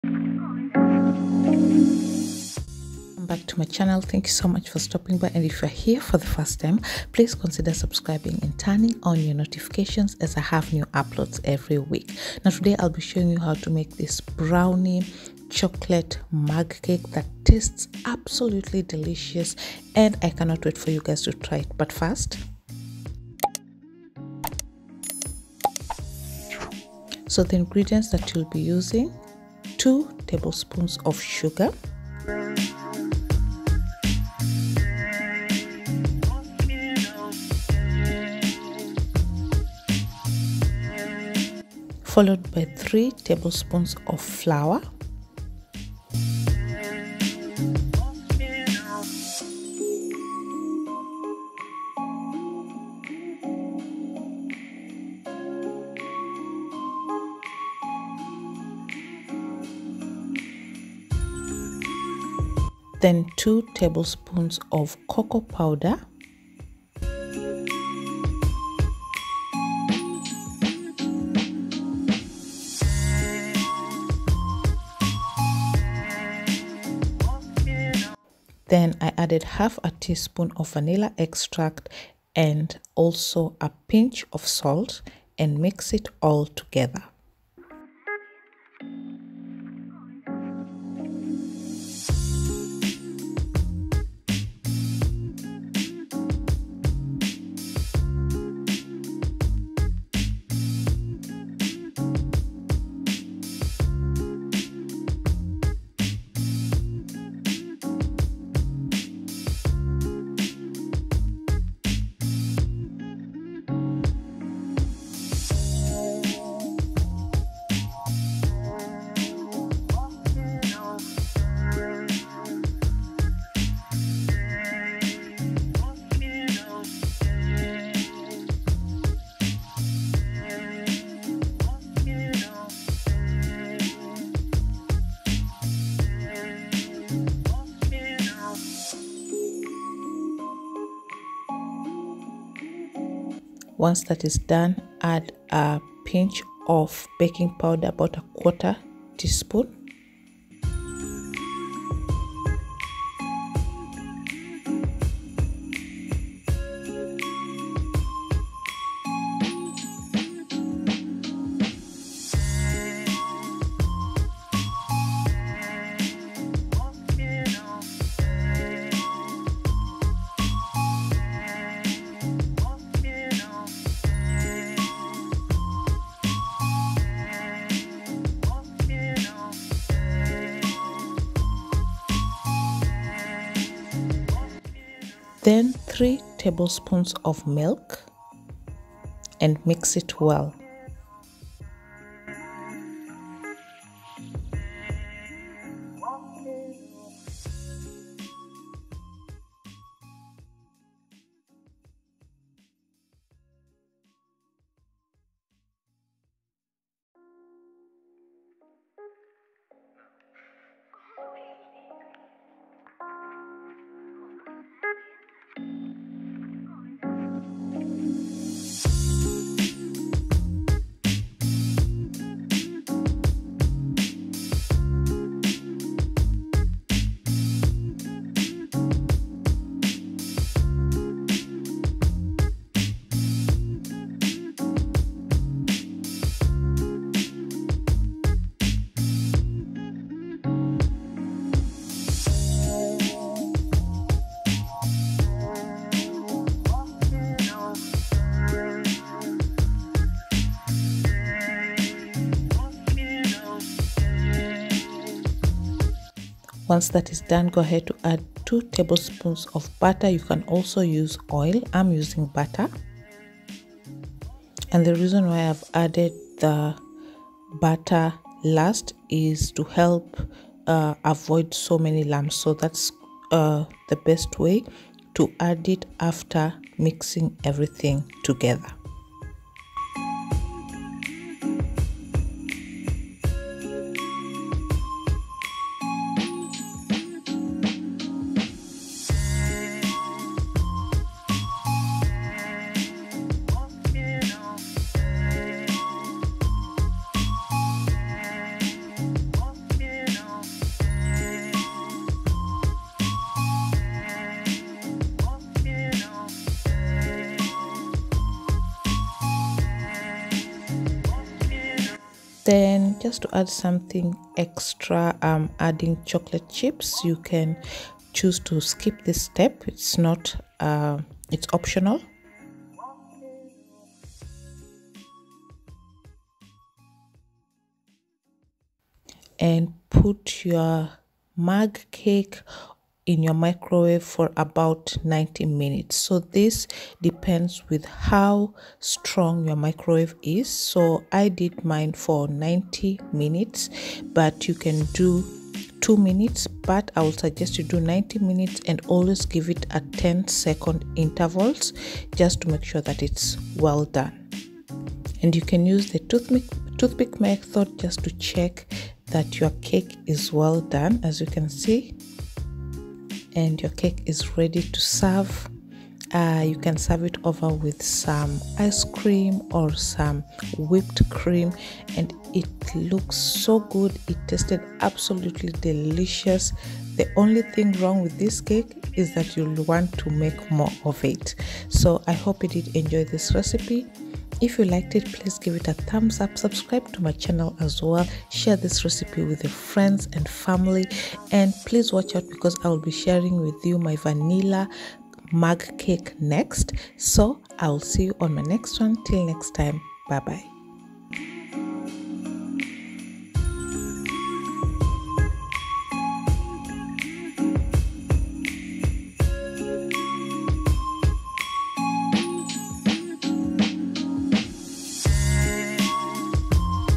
How to make a 2 Minute Chocolate / Brownie Mug Cake || Eggless Microwave Mug Cake.
Mug Cakes make the perfect quick desert and today we will be making a Chocolate one.
INGREDIENTS:
3 Tbspn Flour
2 tbspn Sugar
2 Tbspn Cocoa powder
1/2 Tsp vanilla extract
 pinch of salt
1/4 tsp of baking powder
 Butter , Margarine or oil 2 tbspn
3 tbspn Milk
Chocolate chips (Optional)